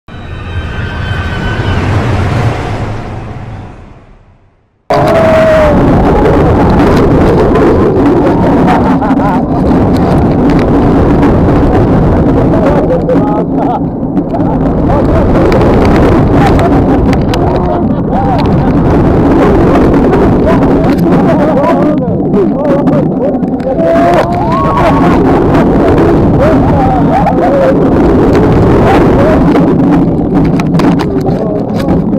SAW The Ride POV FRONT ROW - 1080p GoPro - Thorpe Park - TPB
Take a front row ride on SAW The Ride, the world's first horror movie rollercoaster as you experience a ‘beyond vertical’ drop of 100 degrees from a height of 100ft.

Comment your favourite memory of SAW The Ride!